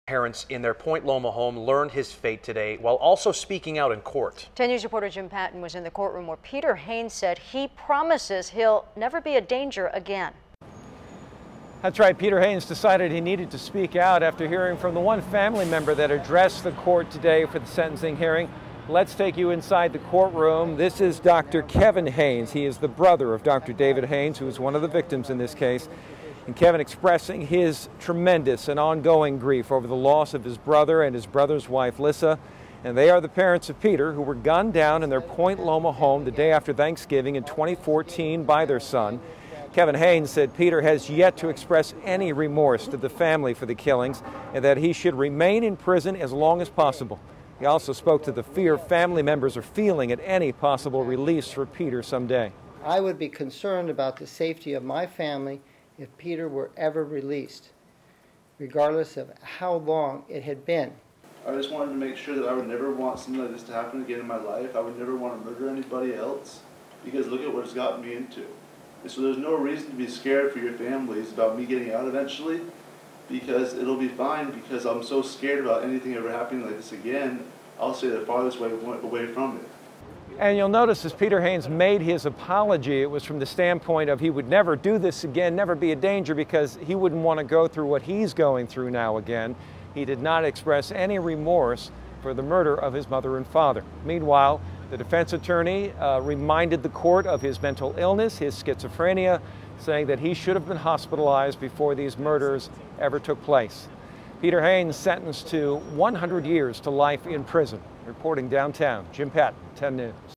Man sentenced 100 years to life for killing parents
Peter Haynes spoke out in court after being sentenced for killing his parents in 2014.